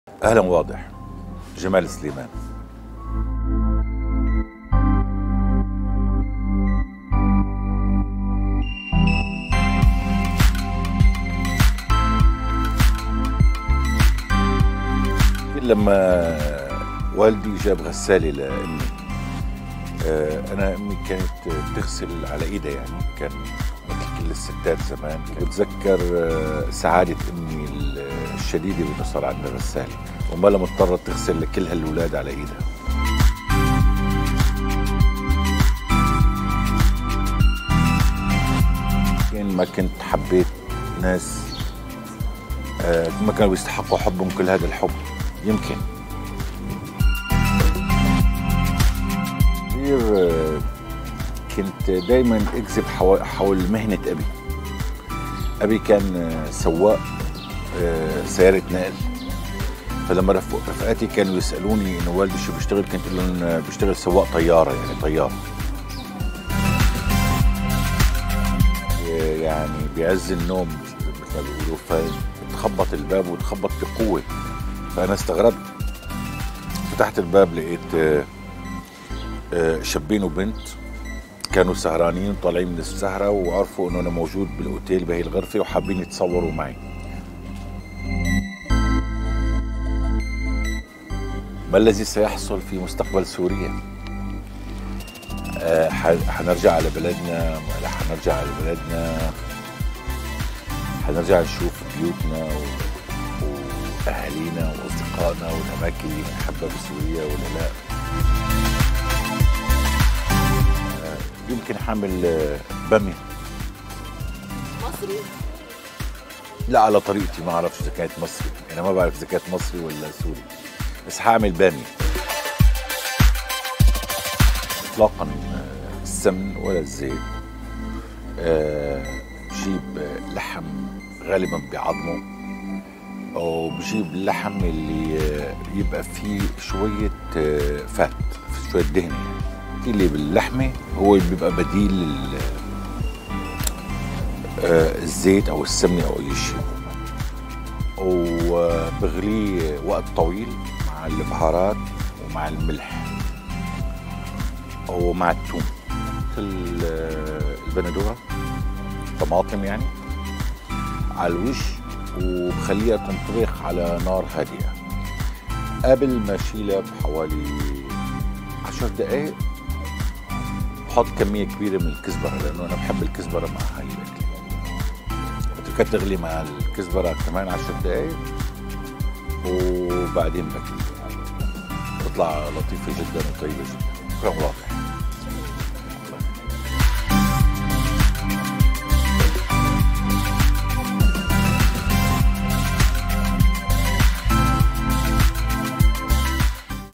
"J'ai aimé des personnes qui ne le méritent pas"
L'acteur syrien Jamal Soliman répond à toutes nos questions. Il nous a surtout donné sa recette de "Bamia"! 😍

Wada7 Original